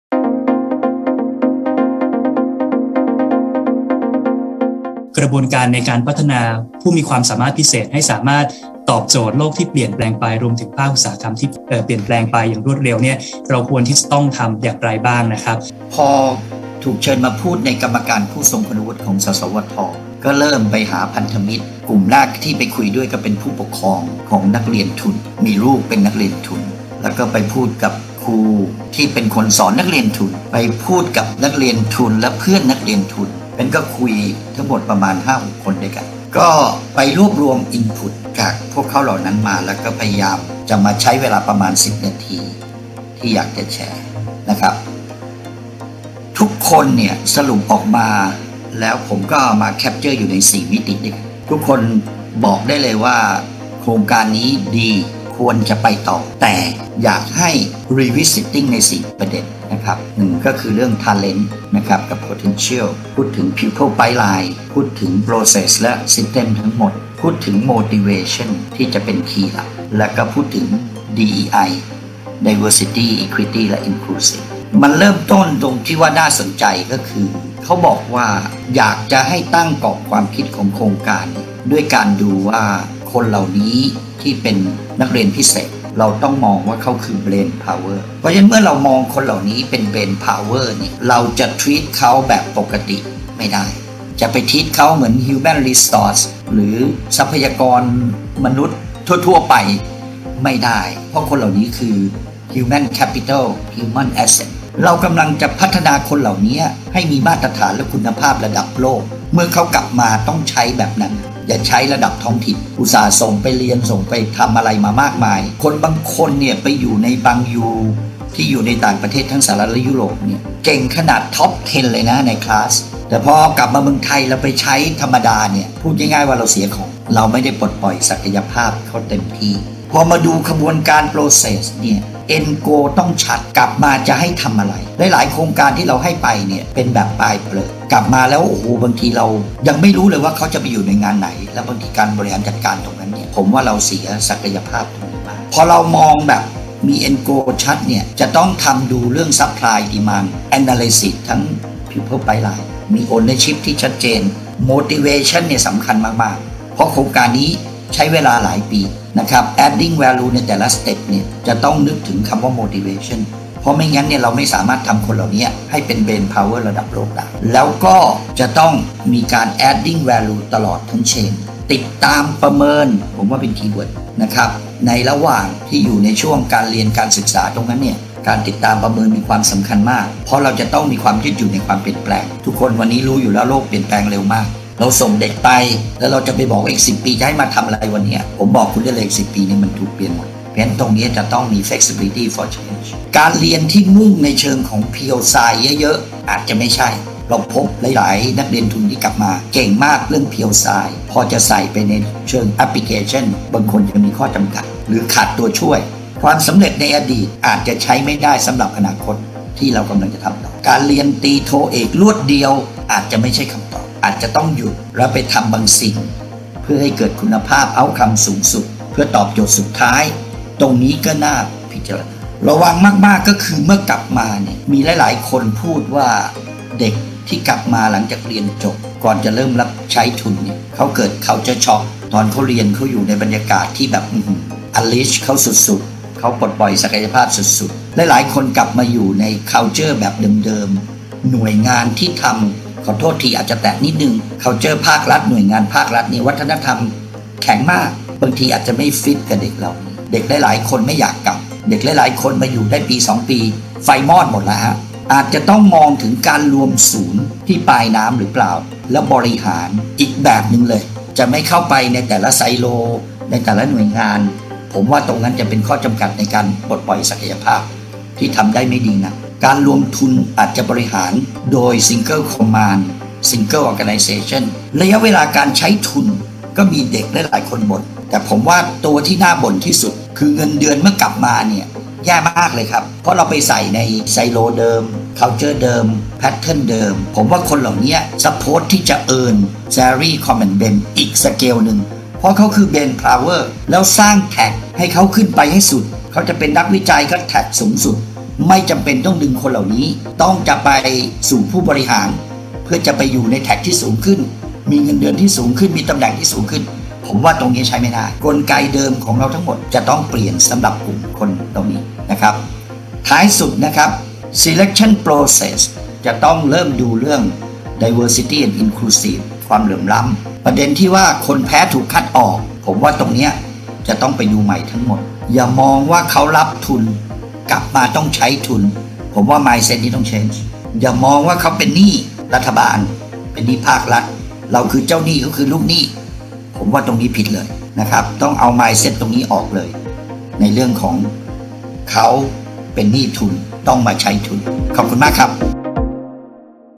ทิศทางการส่งเสริมผู้มีความสามารถพิเศษในยุค 4.0 | ดร.สัมพันธ์ ศิลปนาฎ
Redesigning Future Education การออกแบบการศึกษาแห่งอนาคต
การพัฒนาและส่งเสริมผู้มีความสามารถพิเศษ ที่จะเป็นกำลังสำคัญในการพัฒนาประเทศไปสู่อนาคตด้วยนวัตกรรมทั้งในด้านวิทยาศาสตร์และเทคโนโลยี